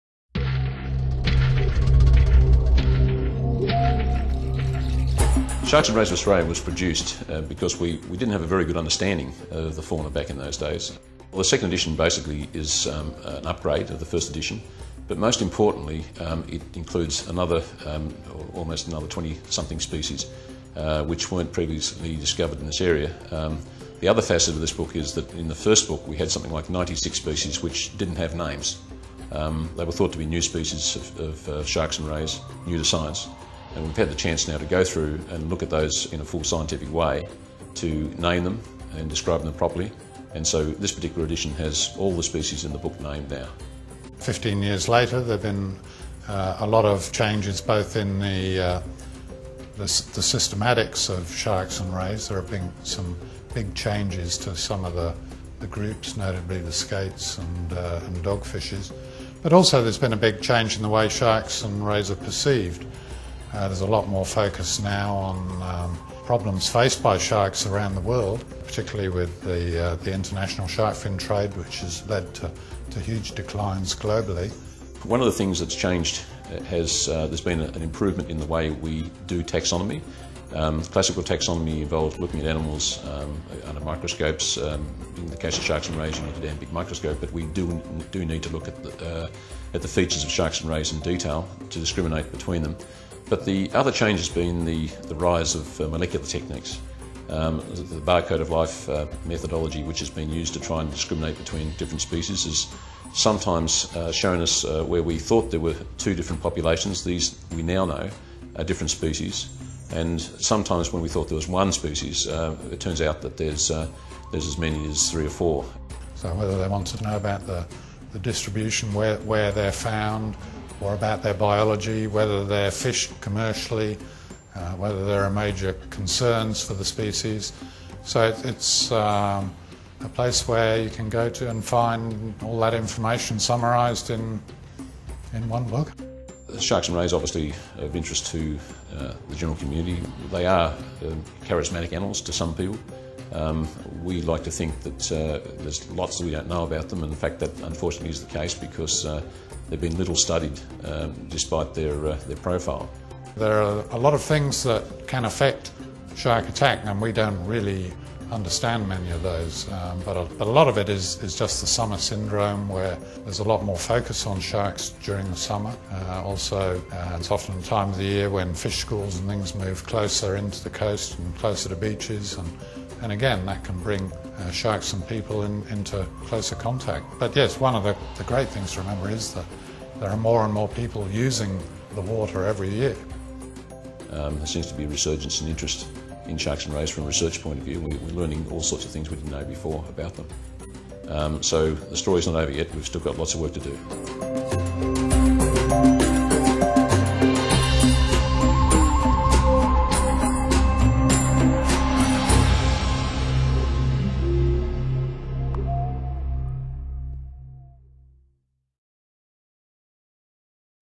Sharks and Rays of Australia – Peter R. Last, John D. Stevens
The waters around Australia, the world's smallest continent, are home to the greatest diversity of sharks and rays on Earth. Fully 100 of these sea creatures (along with their little-known relatives, the chimaerids) have been named or described since the first edition of this book- the biggest revision of the Class Chondrichthyes since the time of Linneaus.

This second edition of Sharks and Rays of Australia brings more than 300 of these species to life in newly commissioned, full-color illustrations.